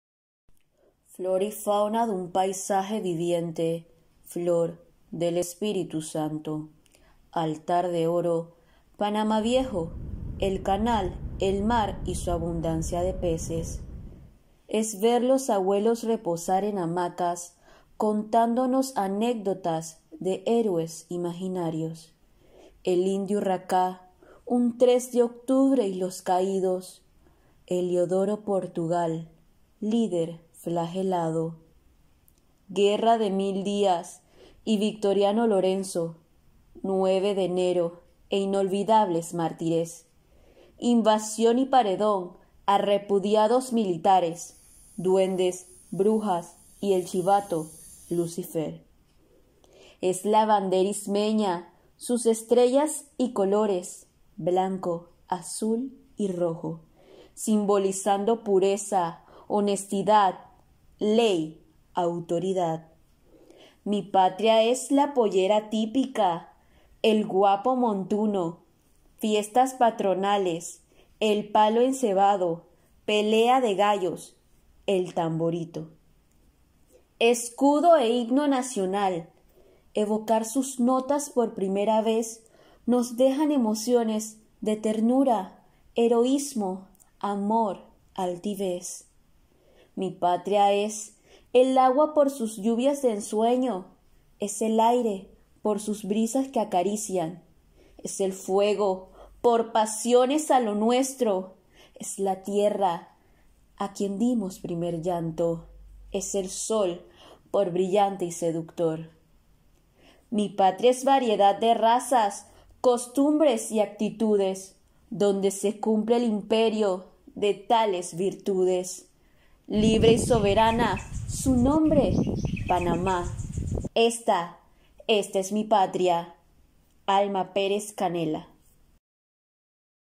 ESTA ES MI PATRIA por Lizzetti Ortega
ESTA ES MI PATRIA propuesta coreográfica de SOLO realizada por la estudiante LIZZETTI ORTEGA en la Materia Técnica de Notación y Diseño de Patrones Escénicos 270b Dictado en el Segundo Semestre de 2021 dictado por la Profesora Julia Olivella en la Carrera de Licenciatura en Bellas Artes con Especializacion en Danzas de la Facultad de Bellas Artes de la Universidad de Panamá.

Poesía elegida: "Esta… Esta es mi patria", de Alma Pérez Canela. Esta poesía es una de las más lindas que he escuchado, todo empezó porque en el colegio me gustaba recitar y de todas las poesías que recite con la que más conecte fue esta, la sentí tan propia que no me costó aprenderla fue más bien como si encontrara las palabras para expresar lo que siento por mi patria, es ese amor por lo nuestro y por la naturaleza de mi Panamá. Lizzetti Ortega